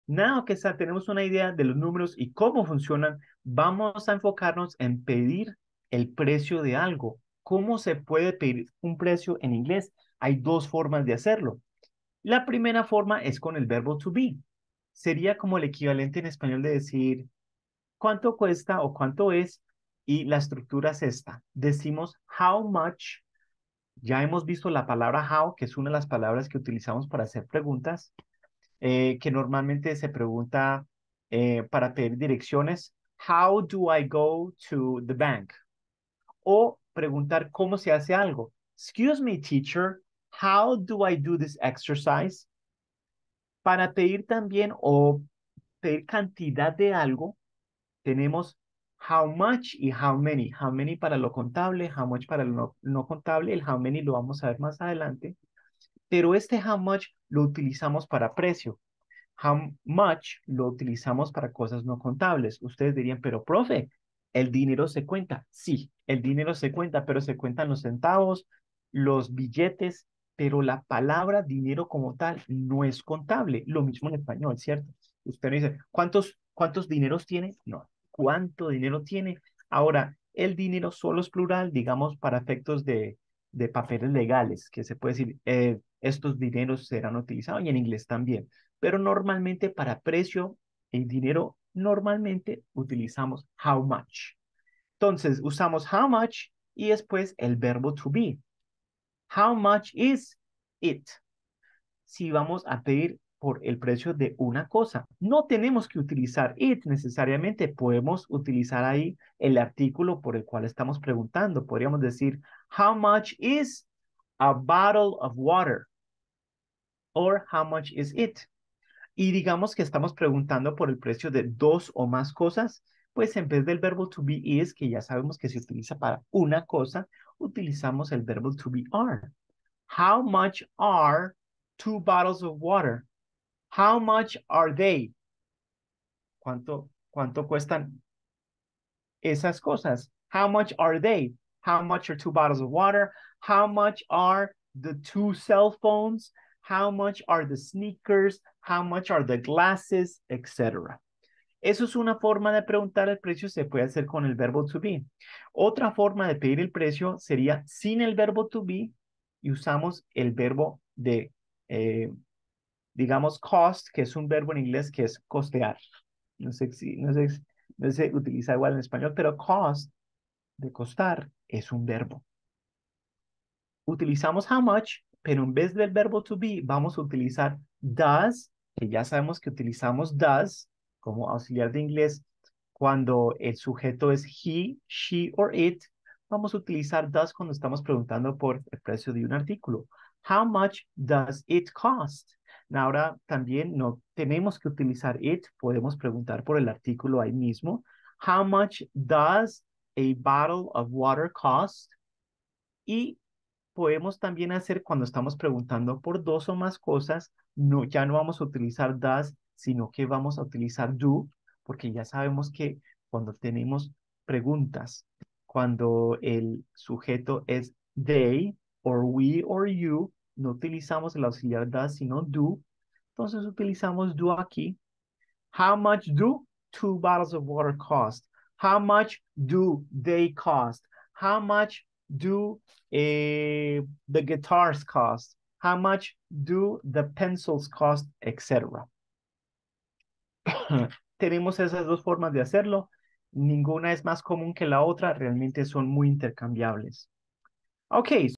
🛍️ Cómo Preguntar el Precio en Inglés: ¡Domina esta habilidad esencial al comprar!
¿Quieres comprar en tiendas extranjeras o en viajes al extranjero con confianza? En este video te enseñamos cómo pedir el precio de un producto en inglés de forma correcta. Aprende las frases y estructuras esenciales para que nunca más dudes al preguntar '¿Cuánto cuesta?'. Dale play y ¡Conviértete en un experto comprador en inglés!